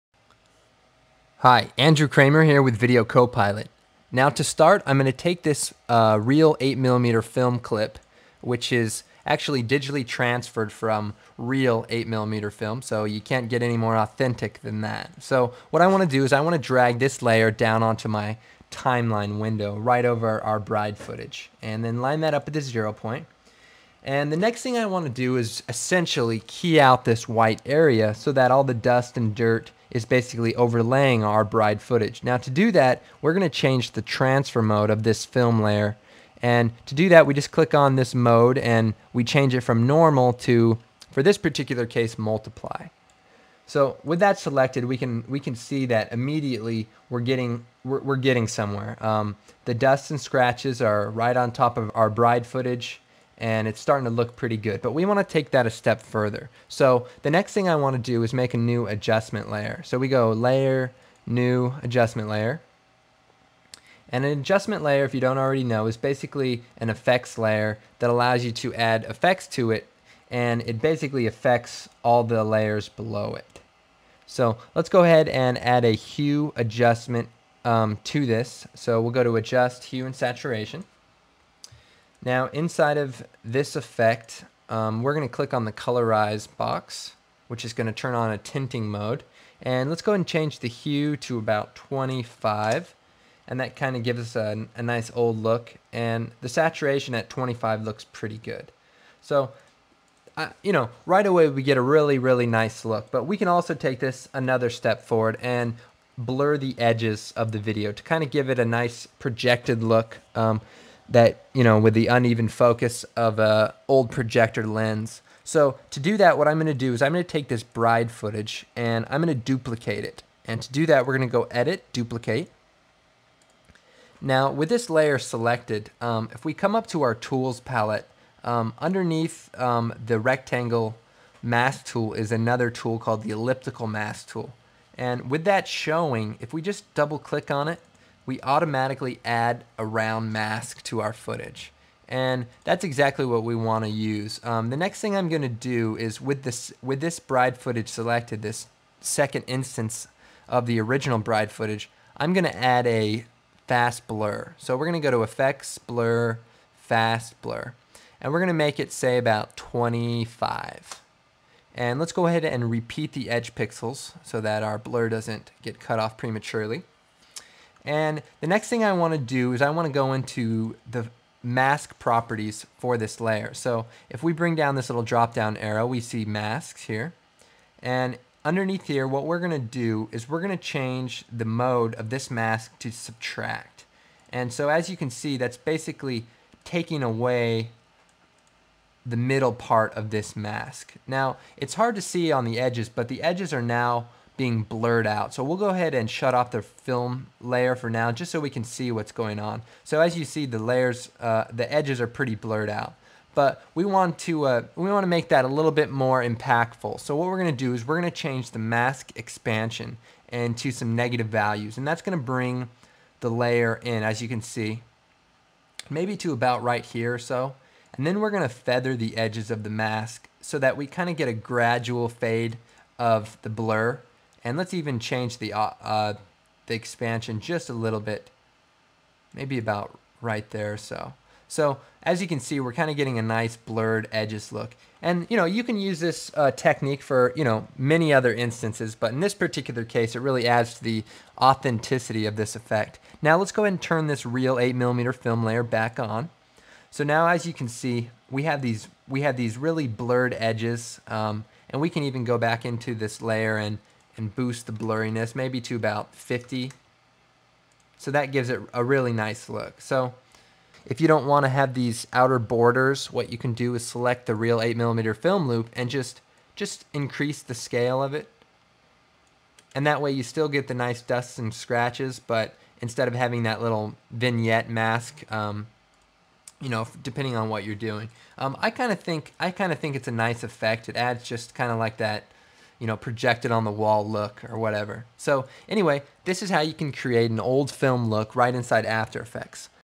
Afte Effects Tutorial 03. Old film look
I am in no way connected to videocopilot.net nor do I receive anything from them for endorsing their products.

Now the tutorials.
Go to videocopilot for much, much, more!
The products to choose from is amazing!

• Create an authentic old-film look
• Introduction to transfer modes and masks